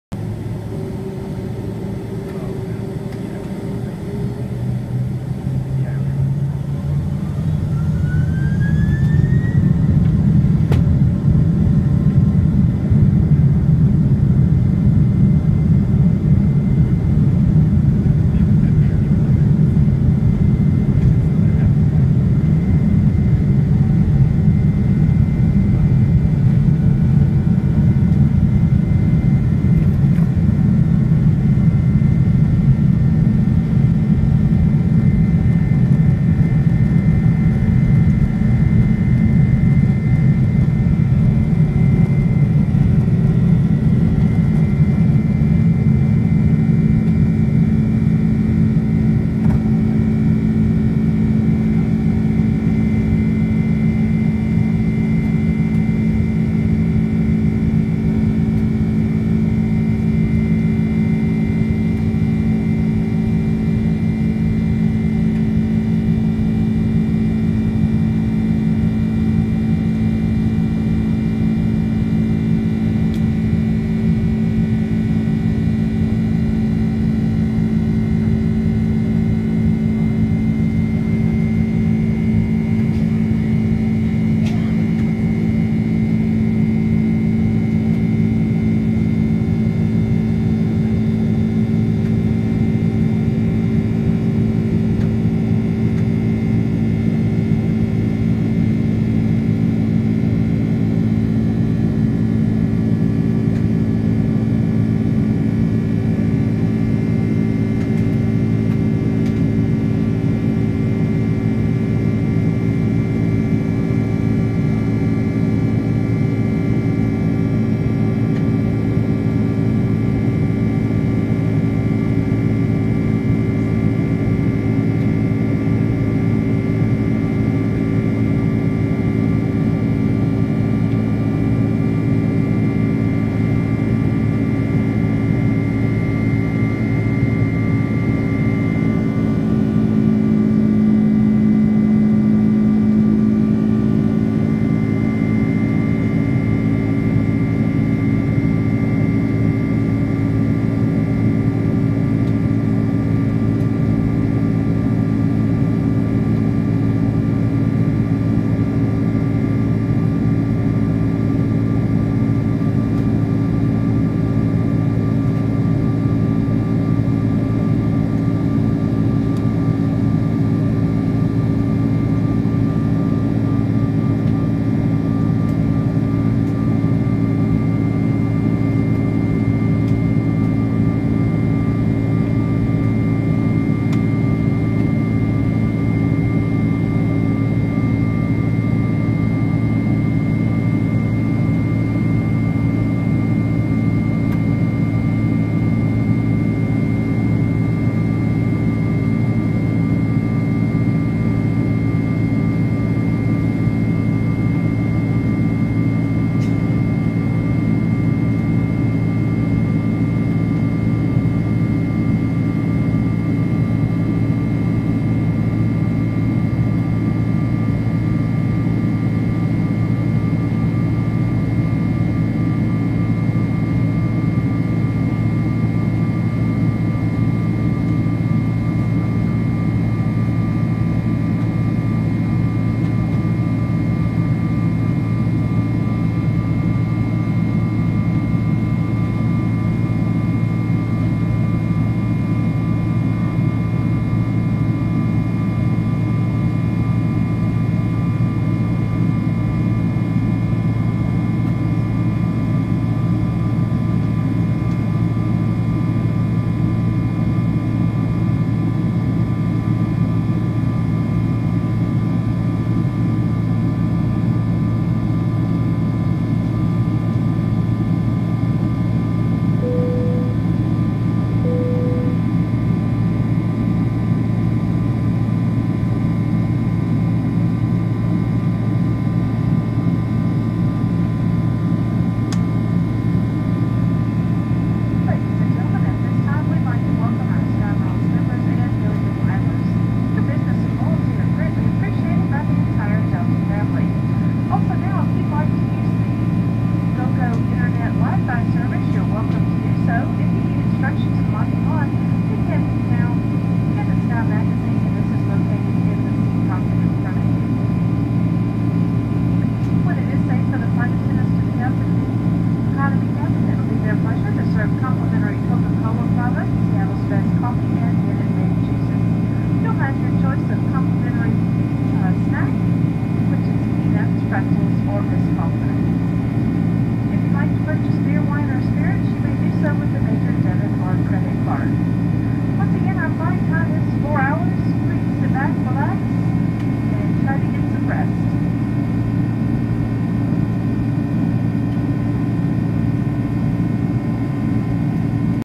Delta 767-300ER Night Takeoff from Los Angeles (LAX-ATL)
Flight 1354 from Los Angeles to Atlanta
Aircraft N1609
Takeoff from Runway 25R.  Note another plane landing on the parallel runway as we began our roll.
Taken from Seat 5A
Business Elite